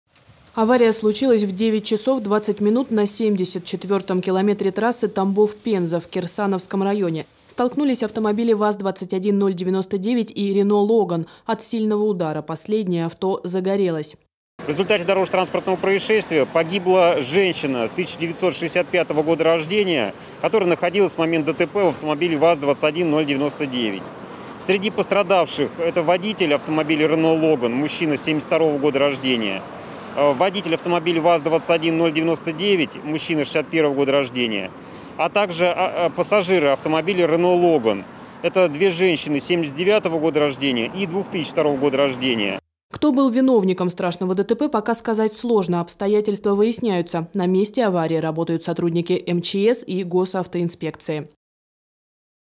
Пять человек пострадали, один погиб: ДТП с возгоранием на трассе в Тамбовской области
Авария произошла 31 августа в 09.20 на 74-м километре трассы Р-208 "Тамбов-Пенза"в Кирсановском районе. По информации ГУ МЧС по Тамбовской области, столкнулись автомобили ВАЗ 21099 и "Reno Logan". От сильного удара последнее авто загорелось.

В результате ДТП пострадали 6 человек. Один из них погиб. Его личность в данный момент устанавливается. 4 участников ДТП доставили в Городскую больницу № 2 имени архиепископа Луки с травмами различной степени тяжести. Детей среди пострадавших нет. Автомобиль "Reno Logan" сгорел полностью.